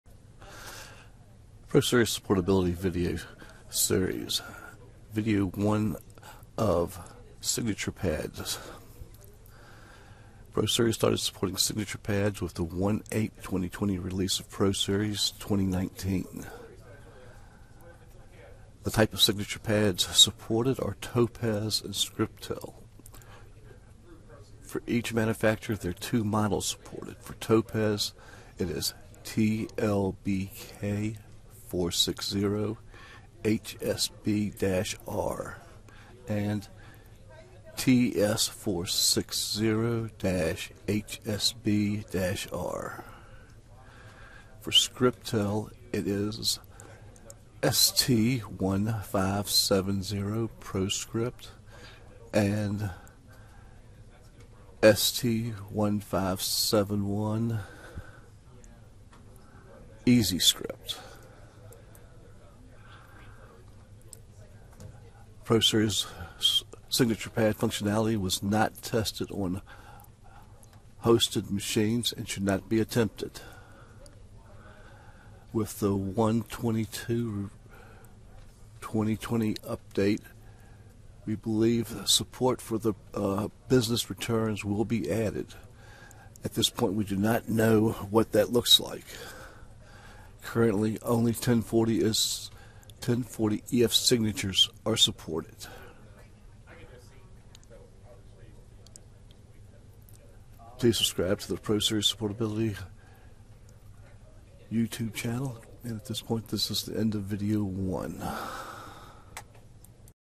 ProSeries Signature pad Basics 1
Basic overview of ProSeries signature pad and what models work.